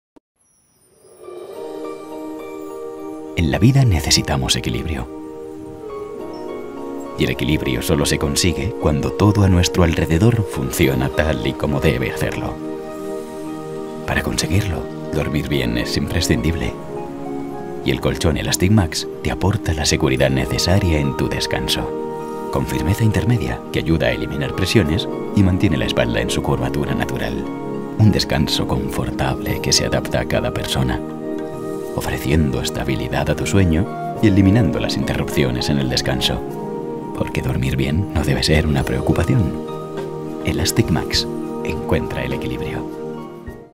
Colchón Elasticmax
Equilibrio perfecto.

Elasticmax te acompaña mientras sueñas, en cada movimiento, en cada giro, cada segundo que pasas en la cama, adaptándose a tu descanso. Sin presiones. Firmeza intermedia que mantiene tu espalda perfectamente alineada cada noche.